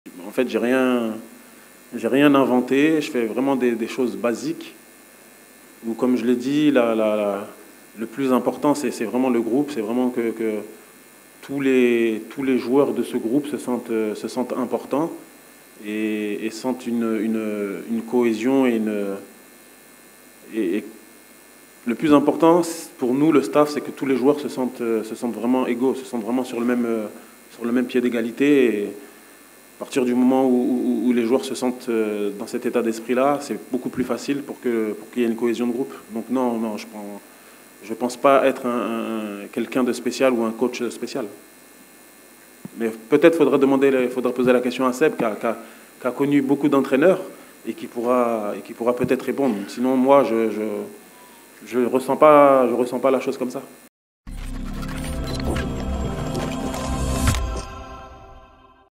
Fae Emerse vu comme " The special one" par un journaliste Nigerian
Quand un journaliste du Nigeria  a demandé au sélectionneur Emerse Faé s’il est « the special one 1 ».